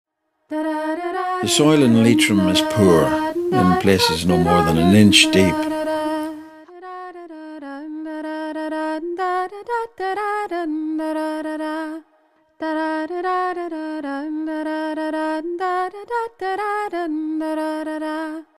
Stephen Rea 😍 - trailer - Iron Mountain Lit. Fest. ‘21 - discussion & readings of John McGahern
Stephen Rea 💝 at *Iron Mountain Literature Festival ‘21 - Beyond Borders*

- trailer from 5-Oct-2021 -

discussion & readings of Leitrim-based writer and novelist John McGahern’s (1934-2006) work and general Stephen Rea related subjects, streamed online on 7-Oct-2021